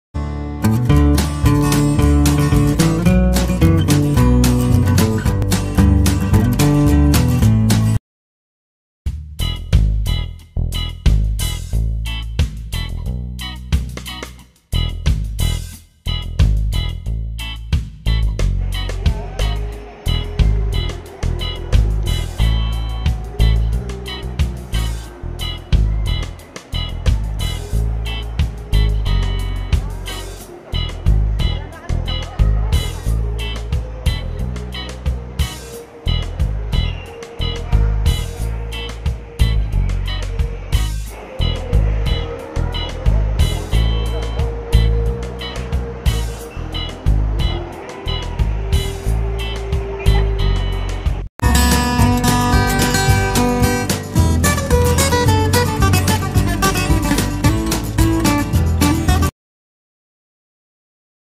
Nag vlog ako kasama si mader ko.
Tuwing heart's day dami mga pakulo Ang sm lalo sa mga background nito like animals, plants at iba pa. kaya panoorin mo lng